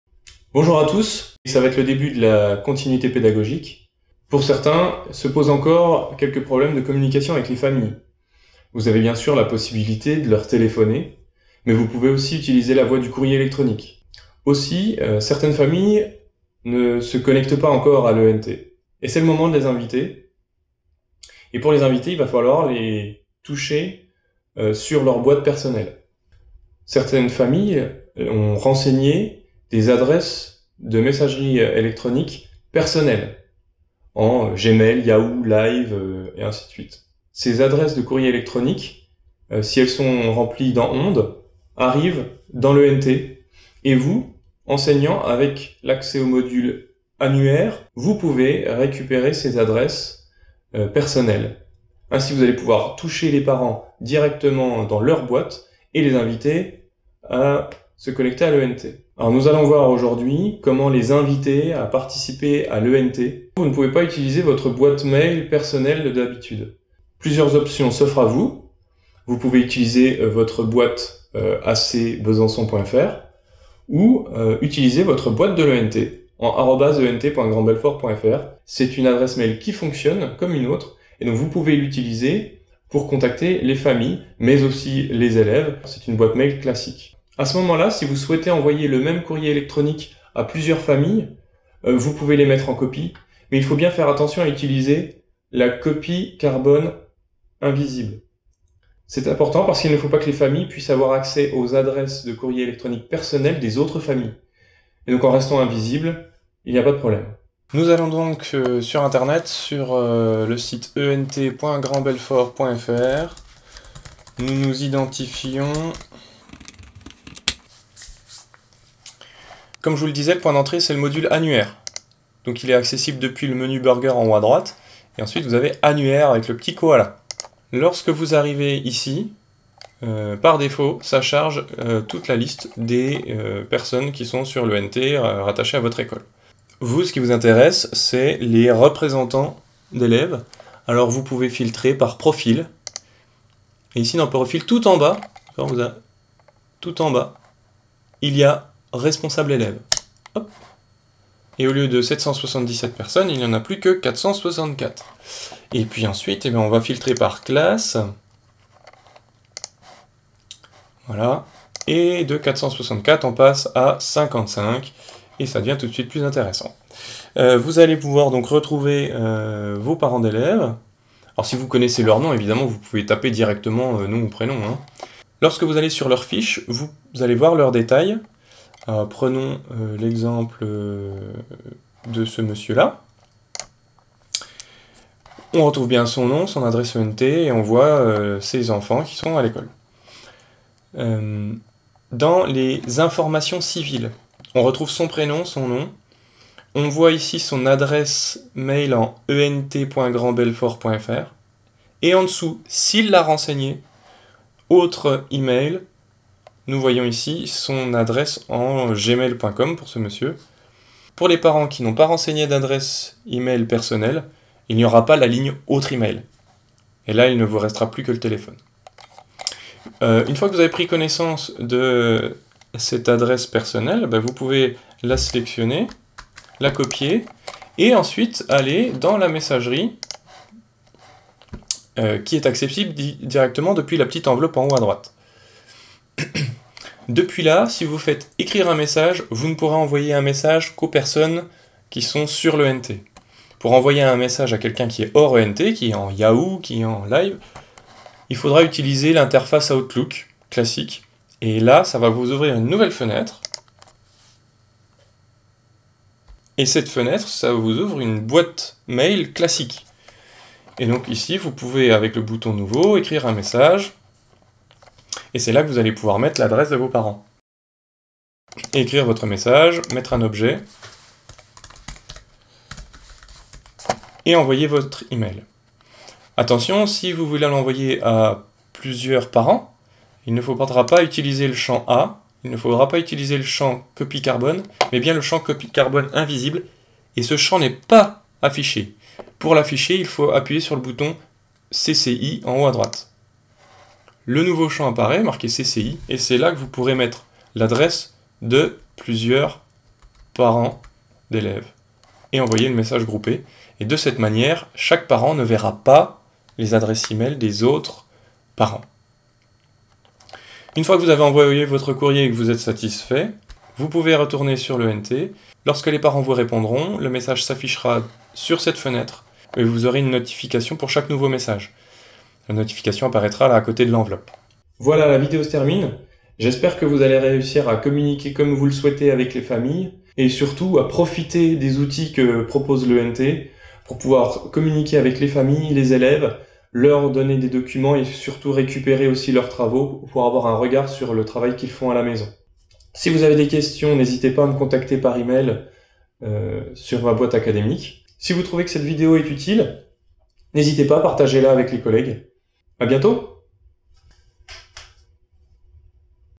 ENT OZE - Contacter les parents boite mail perso, sans boite académique
Voici une procédure pour contacter les parents qui ne sont pas actifs sur l'ENT. Vous pourrez les contacter directement sur leurs boites emails personnelles sans donner votre adresse académique.